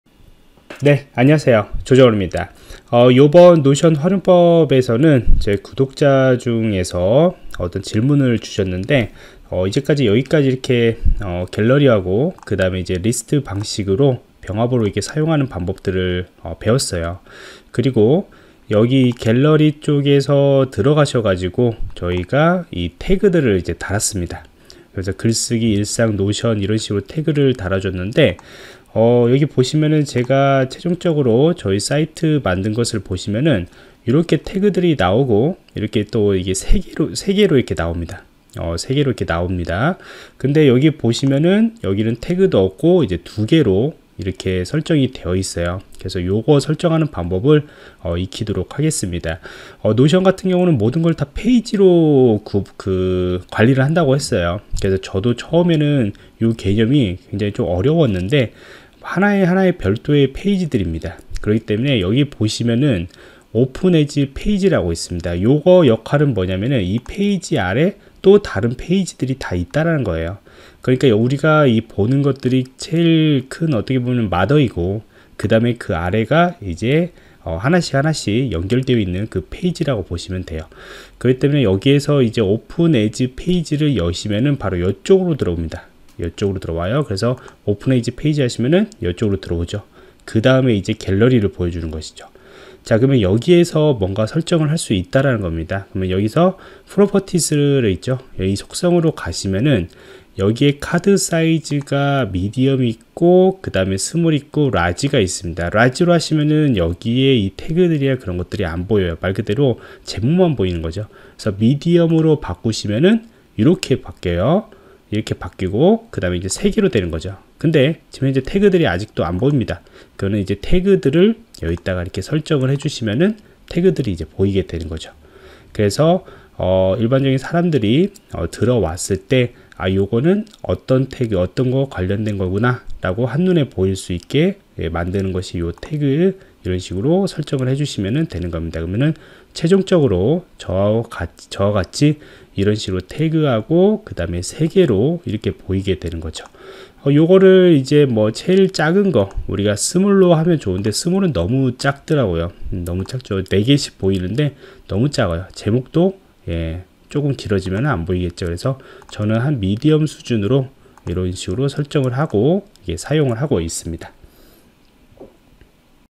노션 (notion) 완벽 활용법 11. 게시판 리스트의 크기 설정 및 태그 활성화
보안프로젝트 ) 에서 제공하는 IT강의입니다. 이번 강의 주제는 "노션 (notion) 완벽 활용법 11. 게시판 리스트의 크기 설정 및 태그 활성화" 입니다.